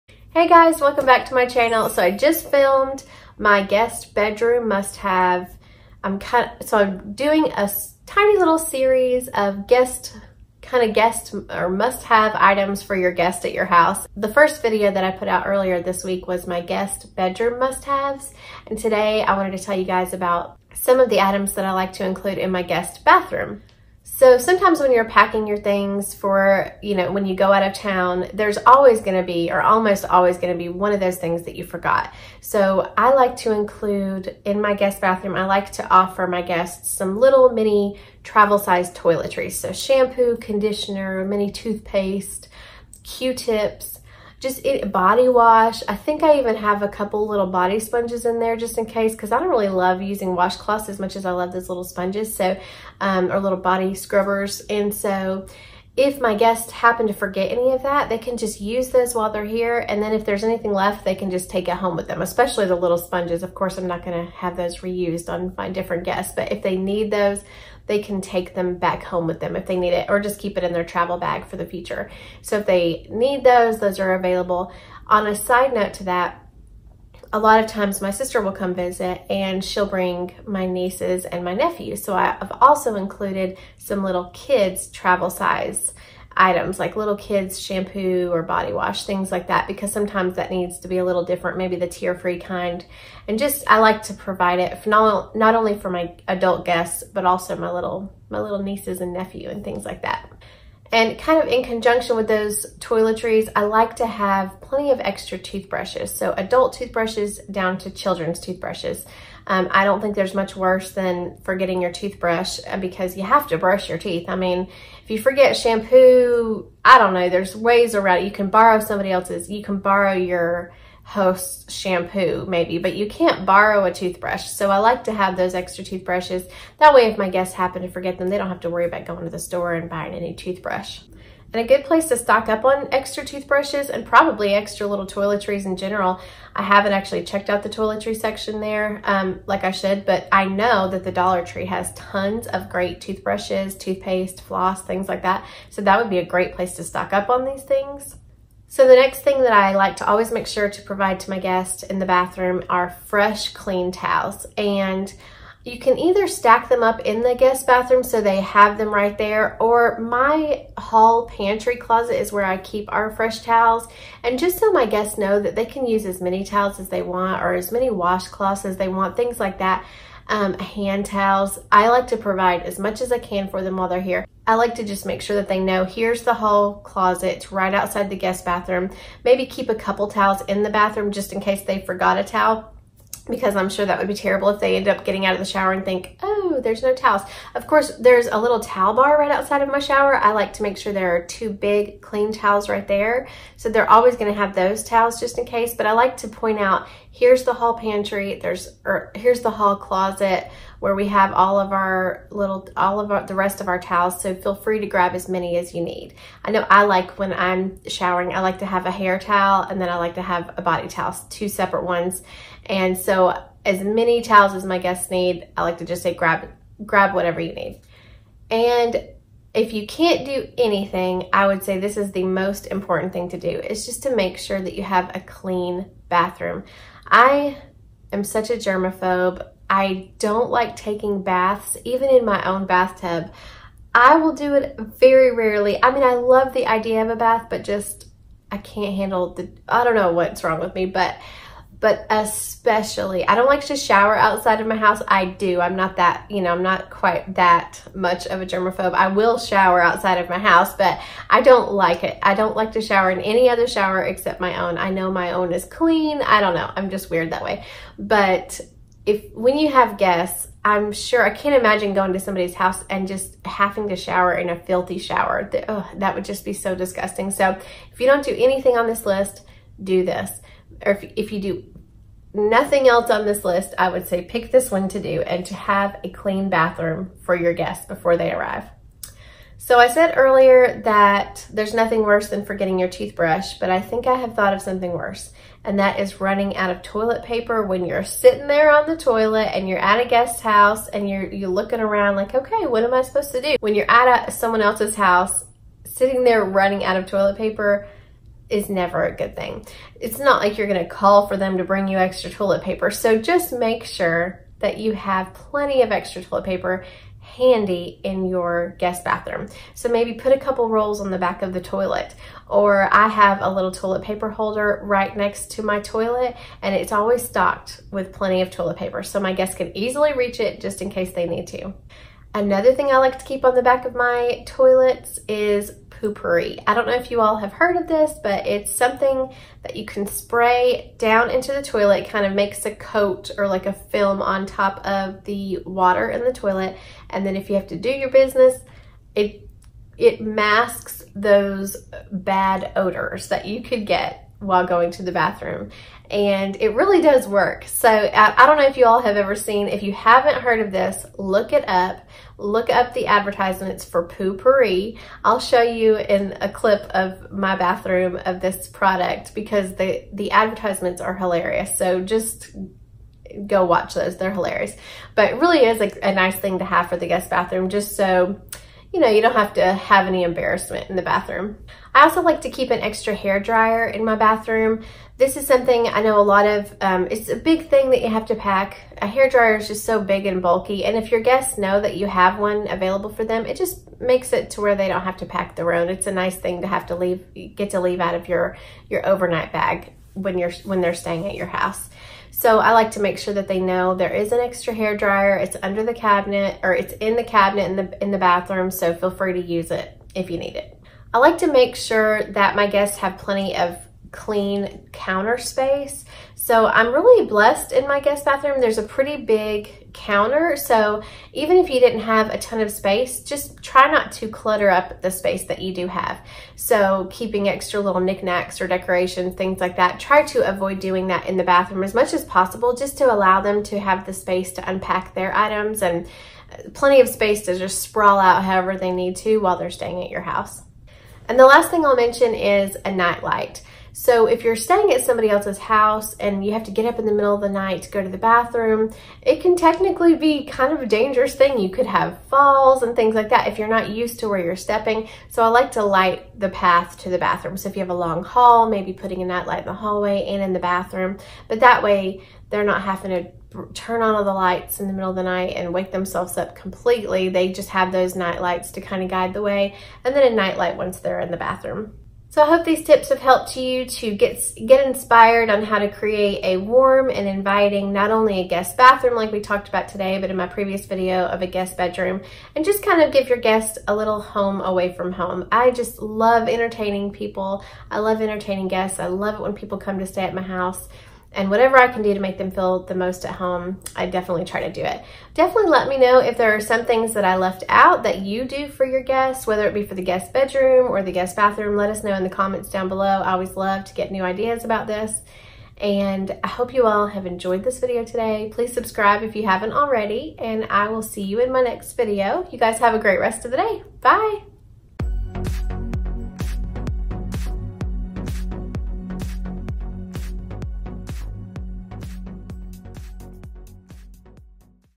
Guest Bathroom Must Haves | Preparing For Guests
Guest Bathroom Must Haves!

Come check out some recommendations to make your guests feel at home when they are staying with you.